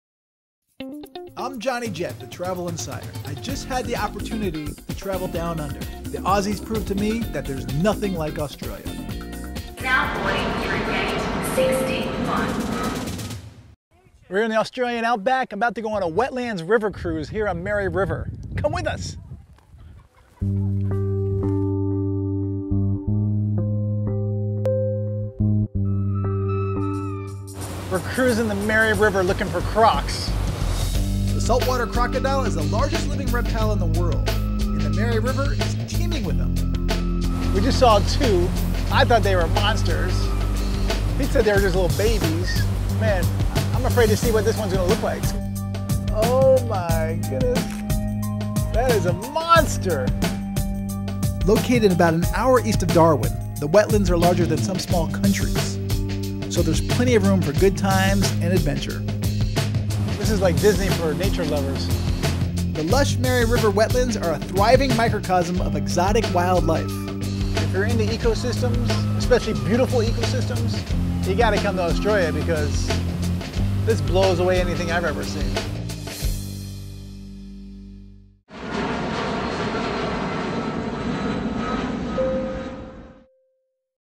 Cruise on the Mary River, Northern Territory, Australia
Getting to The Wildman Wilderness Lodge in Australia's Northern Territory is an adventure in itself.  But the real adventure is when you go on the lodge's cruise on the Mary River. Wow! What an experience -- we saw a ton of prehistoric looking crocodiles, an amazing array of birdlife, and an abundance of wildflowers. Here's our video...